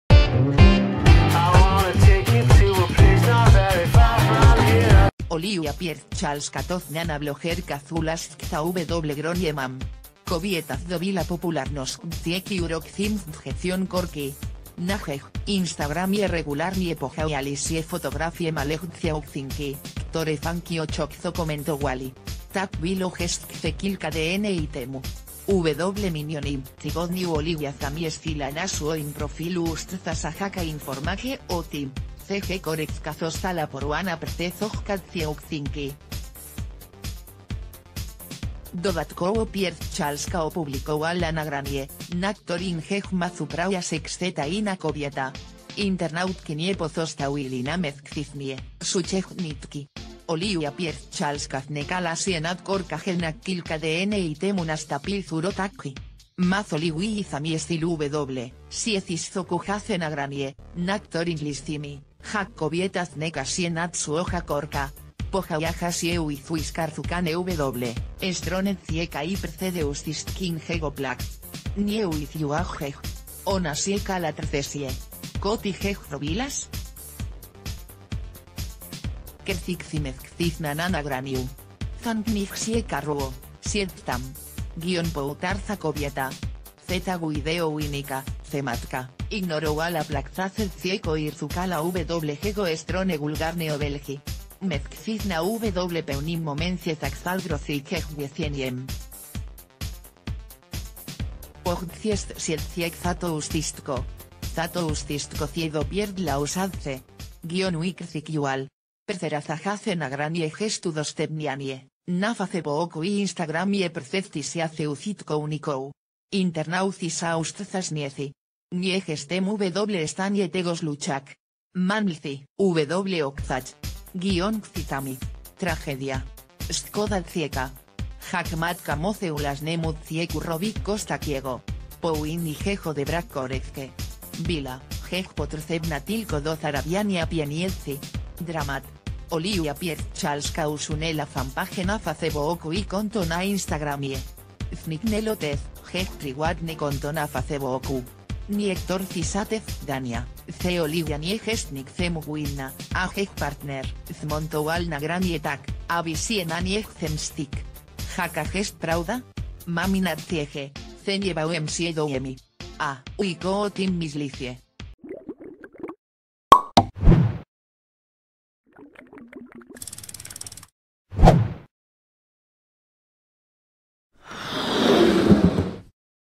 Popularna blogerka znęca się nad swoim dzieckiem? "Zamknij ryj, k*rwo". Jej mąż opublikował szoku...
Popularna blogerka znęca się nad swoim dzieckiem? "Zamknij ryj, k*rwo". Jej mąż opublikował szokujące nagranie